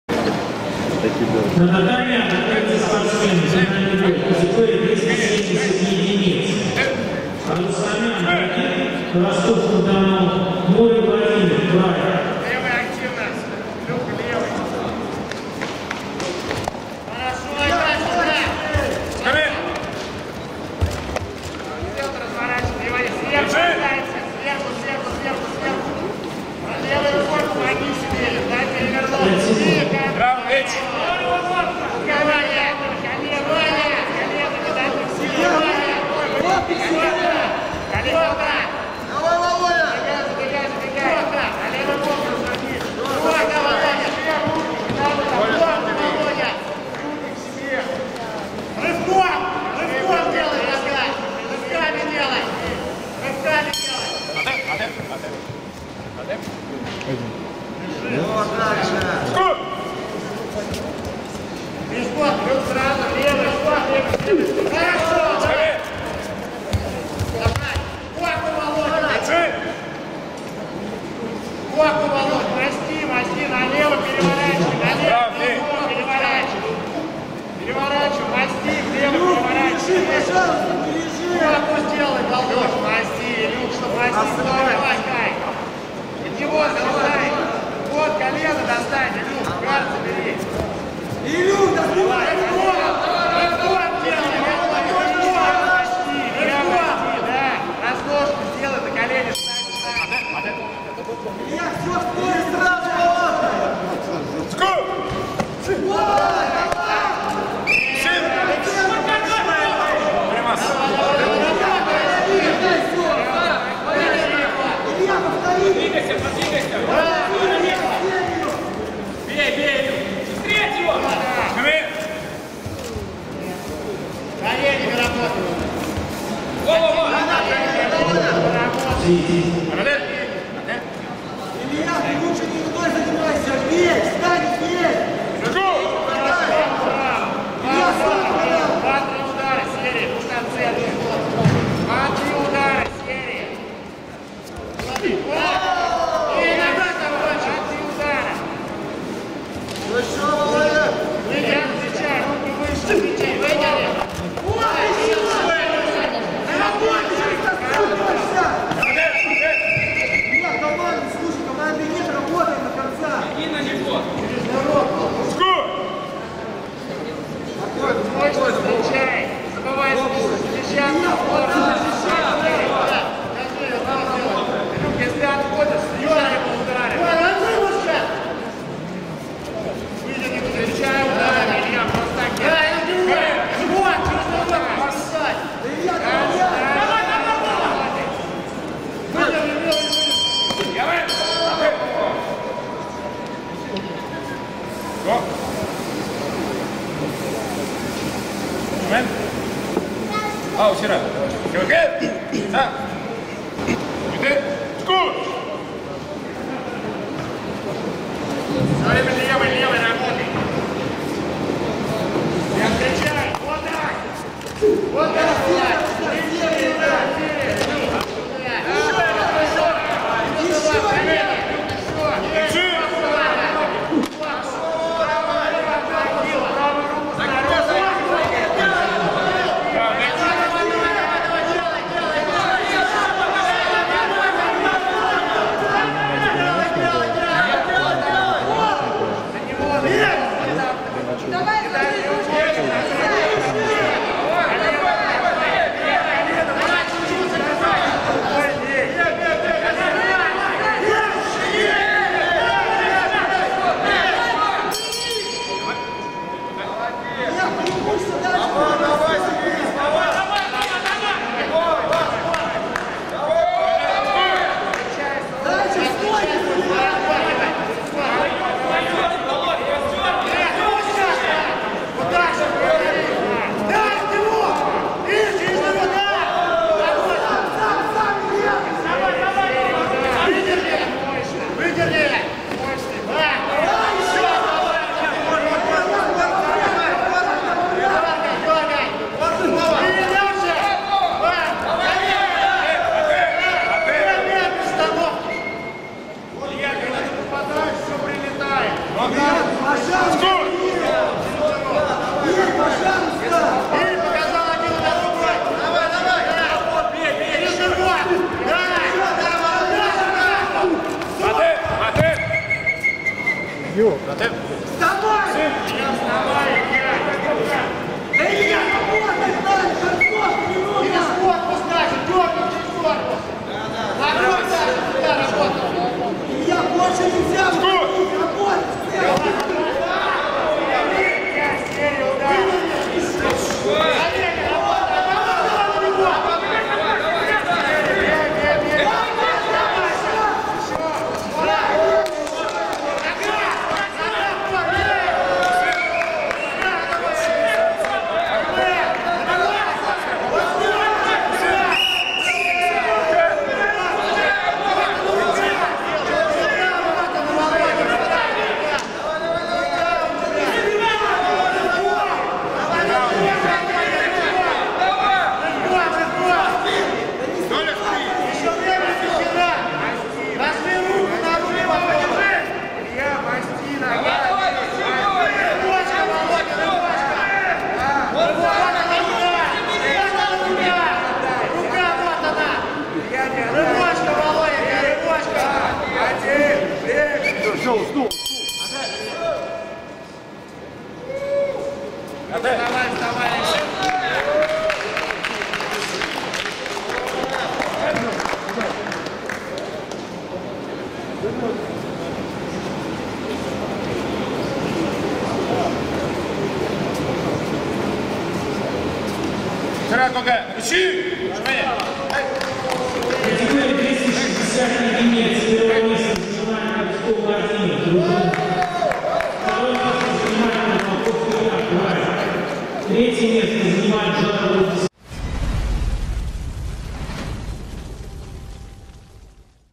Всероссийский турнир по кудо «К-2» 2020 г. - Колпаков vs Редько (Финал, 260 ед)
25 октября во дворце спорта «Динамо» состоялся Всероссийский турнир по кудо «К-2»